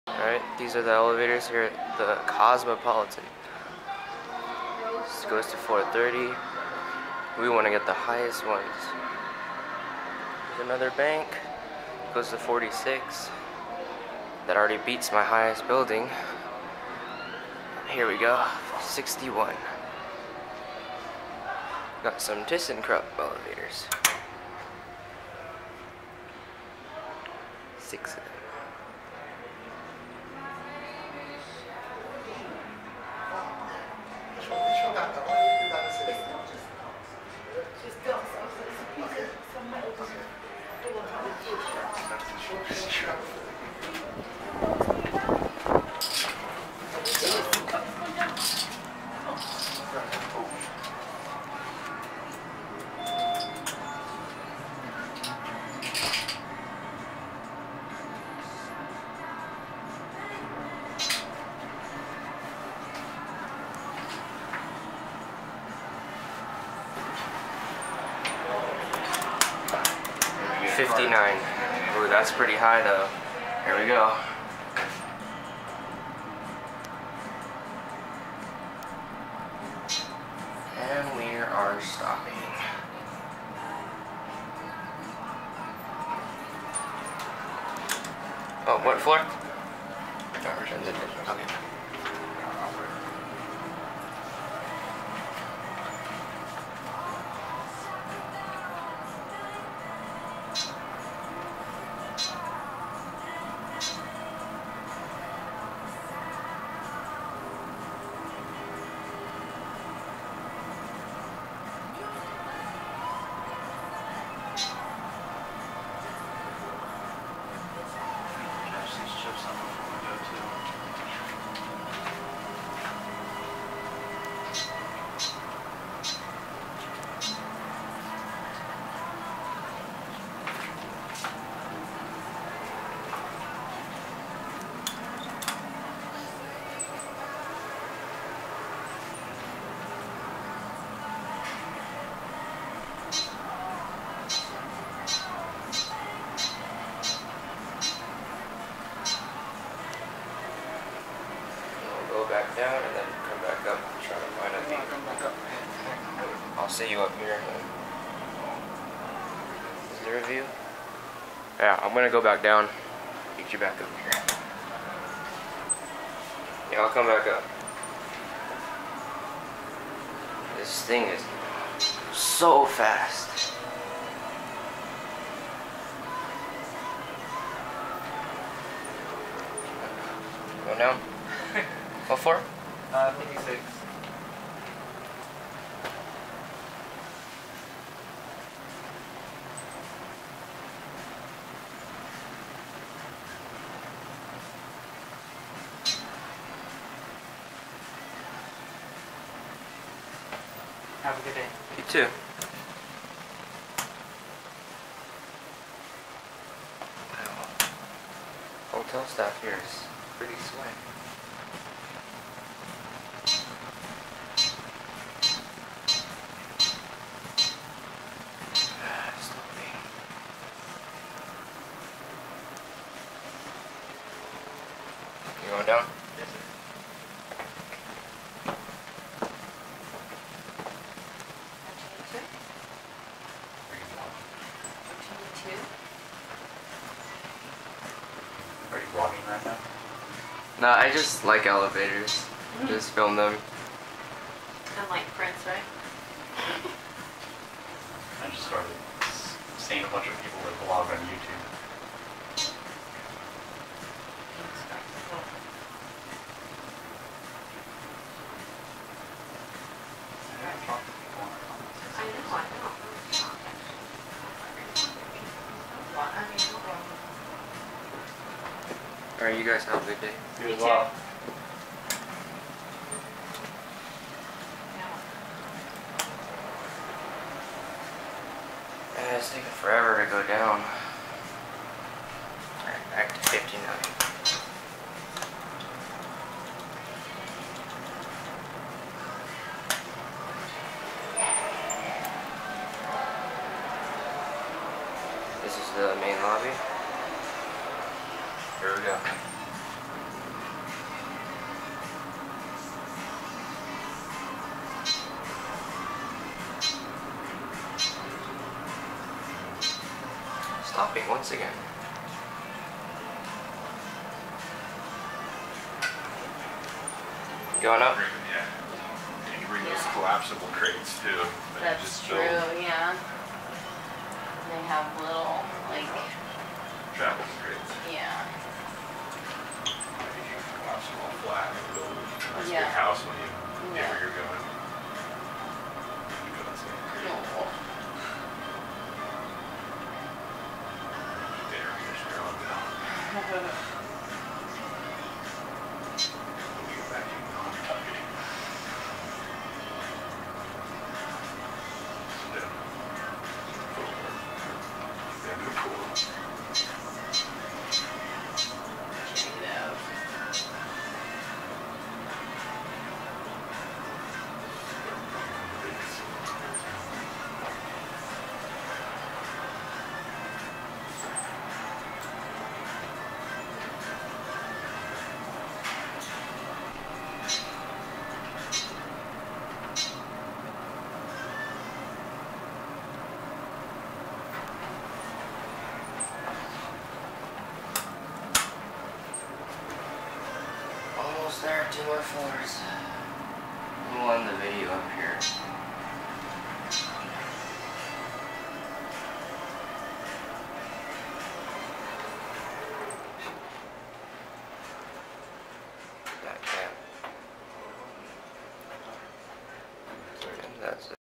ThyssenKrupp Traction Elevators @ The Cosmopolitan-Las Vegas, NV
This was the second tallest building I found while in Vegas, right after the Stratosphere (upload coming soon). This is a very nice and luxurious hotel, and it's actually 61 floors! Unlike the hotels with false numbering, these actually go as high as they're labeled. I was VERY surprised that there was no security at the entrances. For a place like this, you'd think there would be. The Aria as well as the Vdara both had security. I did film some elevators around in those hotels too, but not the main tower elevators.